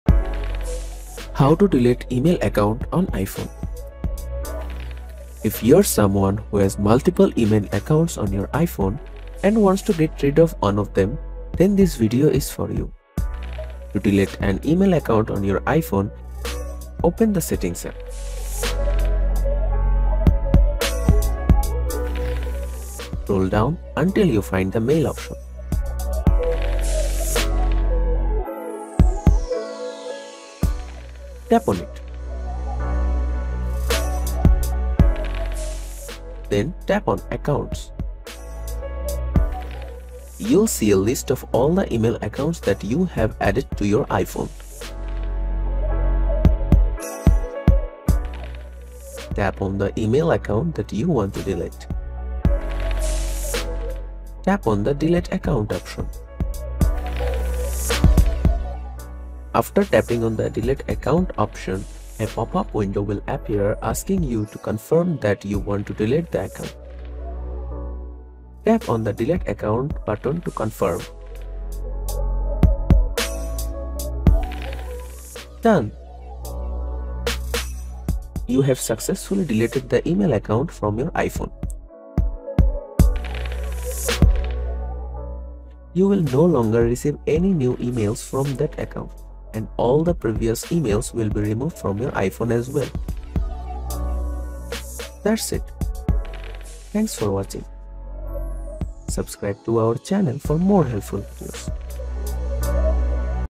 how to delete email account on iphone 2025 [ Easy Way ]
how to delete email account on iphone 2025

This tutorial explains how to delete an email account from an iPhone. These instructions apply  and later, but the steps are similar for all recent versions of the Apple mobile operating system.